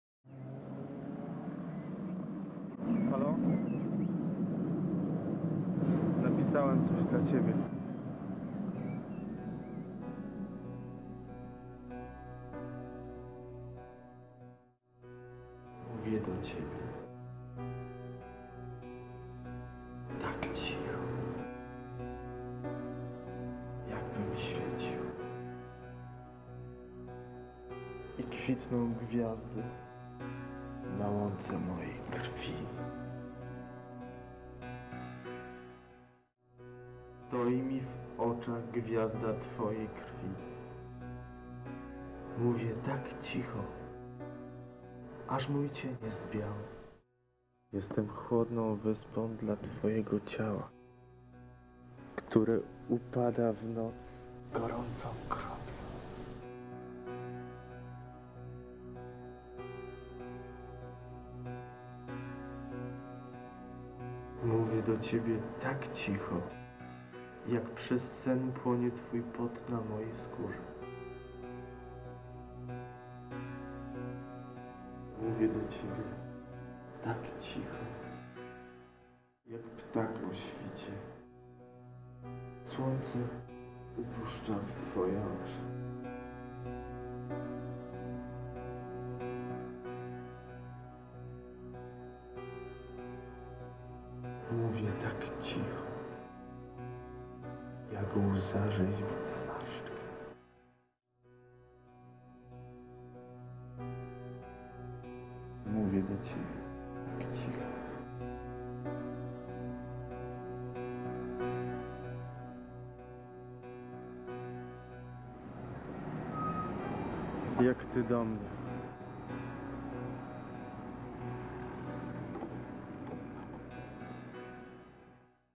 Rafał Wojaczek - "Mówię do ciebie cicho".
Wystąpili: Piotr Drążkiewicz, Agata.
Realizacja: Remigiusz Krela.
Muzyka: Ankh.

"Mówię do ciebie cicho", 11/12. VI. 1966r.

Mówię do ciebie tak cicho jakbym świecił
I kwitną gwiazdy na łące mojej krwi
Stoi mi w oczach gwiazda twojej krwi
Mówię tak cicho aż mój cień jest biały

Jestem chłodną wyspą dla twojego ciała
które upada w noc gorącą kroplą
Mówię do ciebie tak cicho jak przez sen
płonie twój pot na mojej skórze

Mówię do ciebie cicho jak ptak
O świcie słońce spuszcza w twoje oczy
Mówię tak cicho
Jak łza rzeźbi zmarszczkę

Mówię do ciebie tak cicho
jak ty do mnie